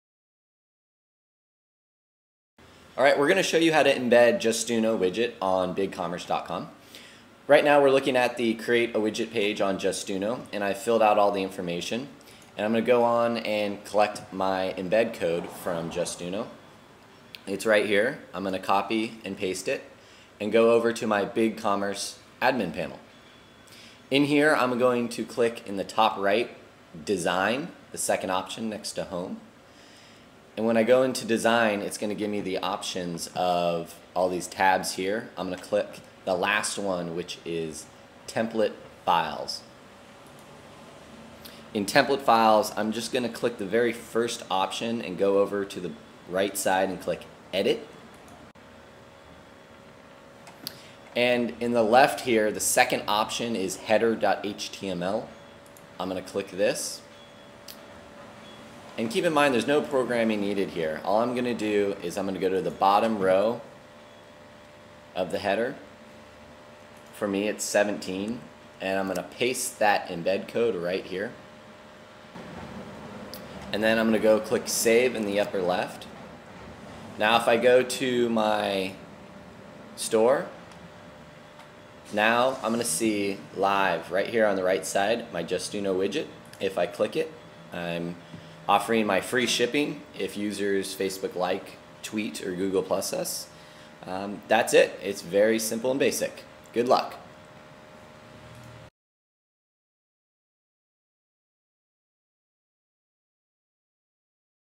BigCommerce-Install a social media widget to get Facebook Fans
BigCommerce.com is a fantastic shopping cart solution for any size online retailer. has created a real time promotion widget to help merchants get facebook likes, tweets, +1's and increase sales.  It's free and takes minutes to install!